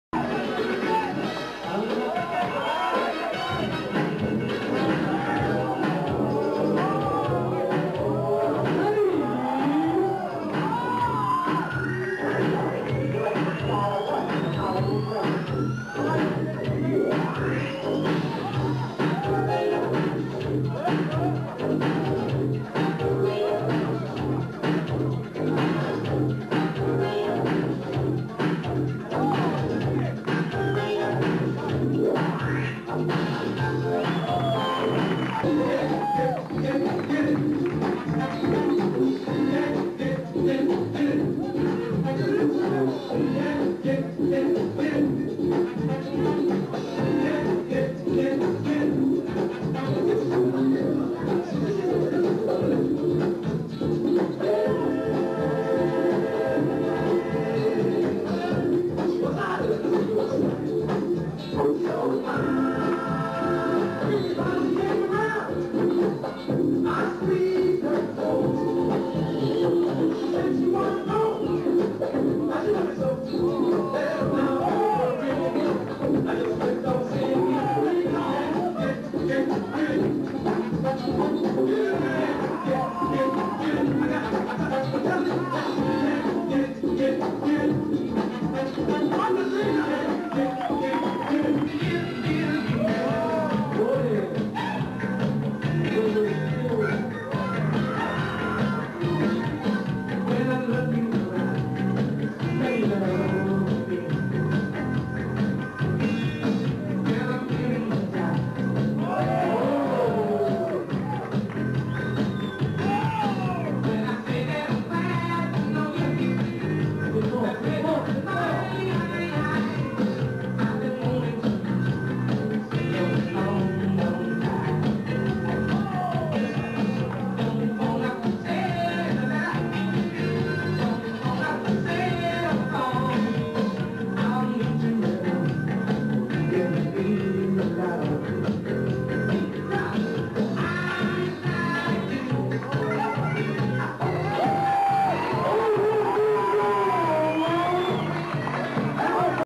Sally Sly
Sally at MCM Cafe. He was DANGEROUS here. His isolations and muscle control are monstrous. Kev